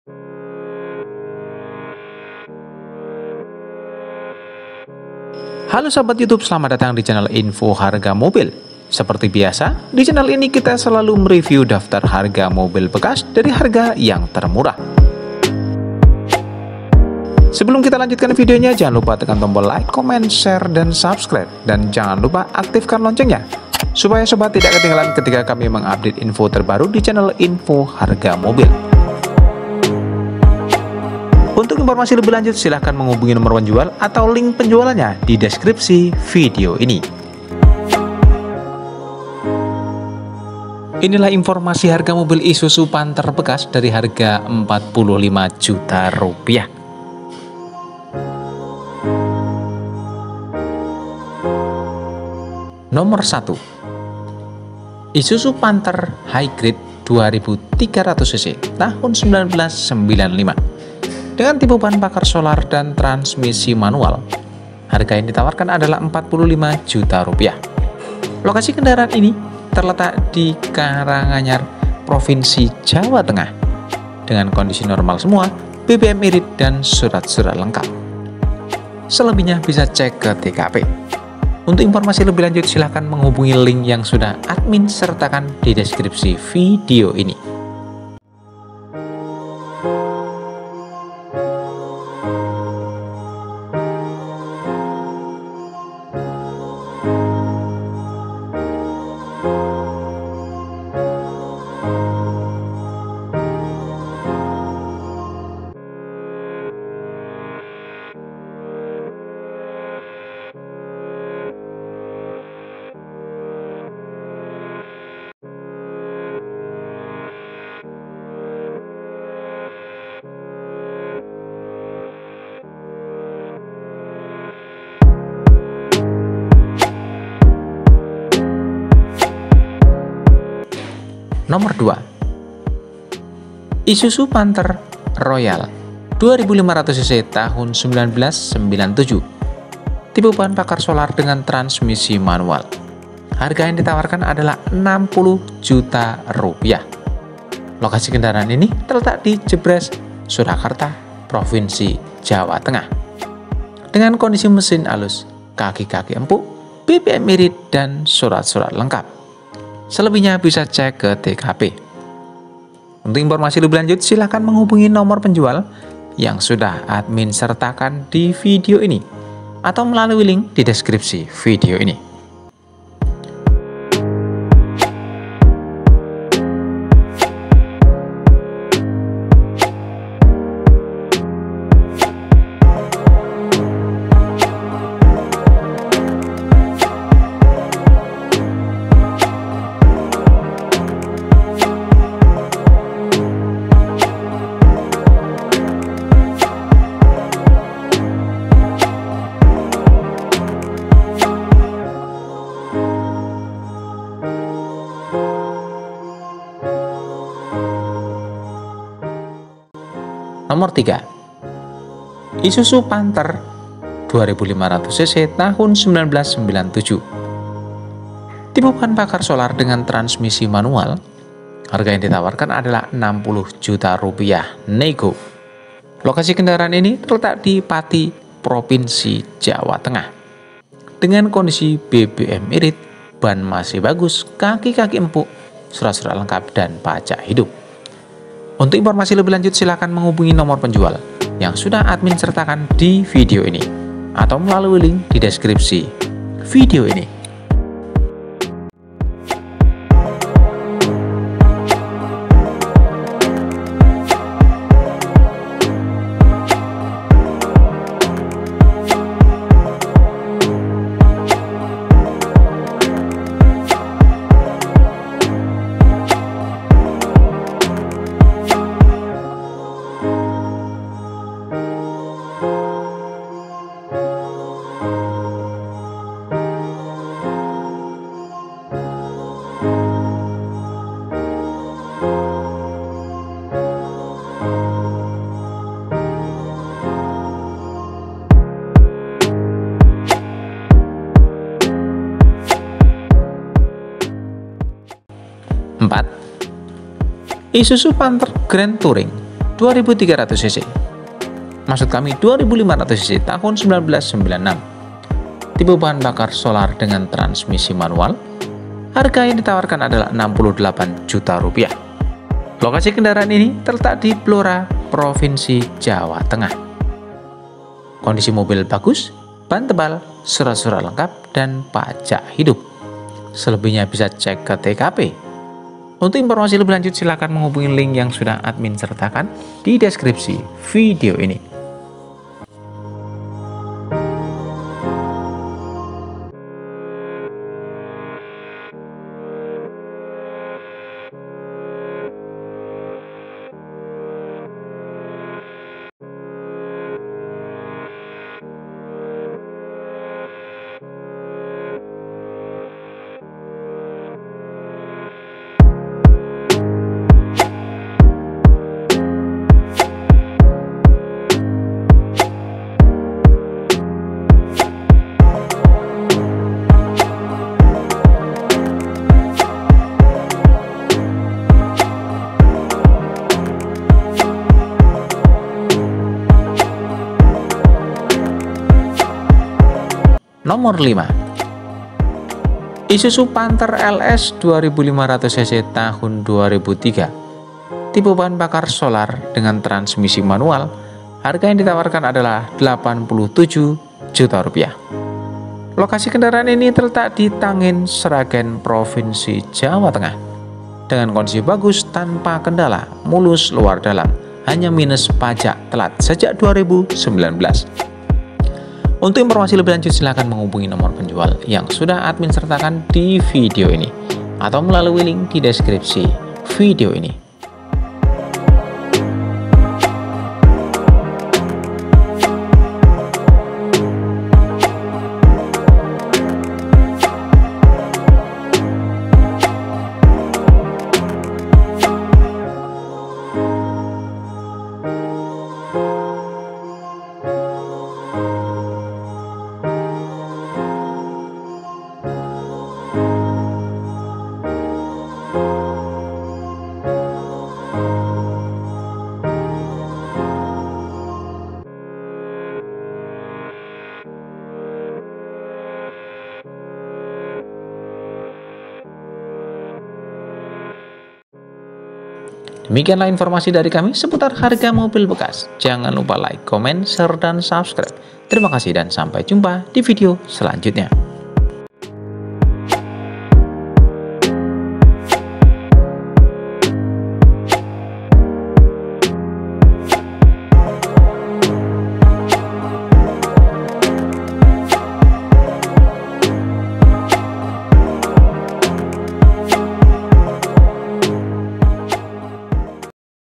Info Harga Mobil Isuzu Panther Murah Mudah di Rawat Harga 45 Juta
ASSALAMUALAIKUM WR.WB. TERIMAKASIH ATAS KUNJUNGANNYA DI CHANEL INI. MARI KITA SIMAK REVIEW DAFTAR HARGA HARGA MOBIL BEKAS

1. ISUZU PANTHER HI GRADE 2300CC 95
HP : -

2. ISUZU PANTHER ROYALE 2500CC 97

3. ISUZU PANTHER 2500CC 97

4. ISUZU PANTHER GRAND TOURING 2300CC 96
HP : -

5. ISUZU PANTHER LS 2500CC 2003

Terimakasih dan sampaijumpa di video selanjutnya.